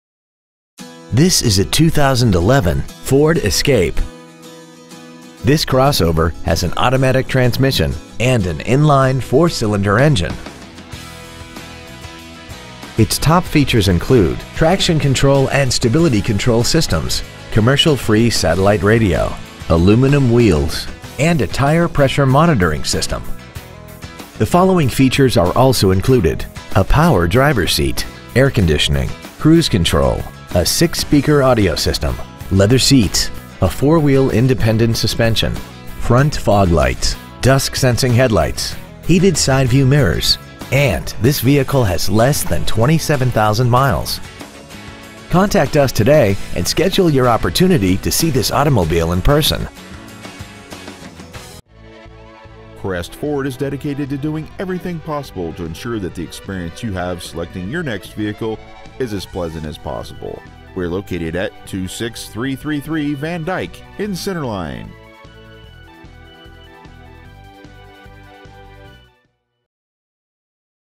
Pre-Owned 2011 FORD ESCAPE Center Line MI
We have been honored to serve the Center Line MI area , we promise that your experience at our dealership will exceed your expectations !

 Year : 2011
 Make : FORD
 Model : ESCAPE
 Engine : 2.5L I4 16V MPFI DOHC
 Trans . : 6-SPEED AUTOMATIC
 Exterior : INGOT SILVER METALLIC
 Miles : 26,935
 Interior : CHARCOAL BLACK
 Stock : F21615L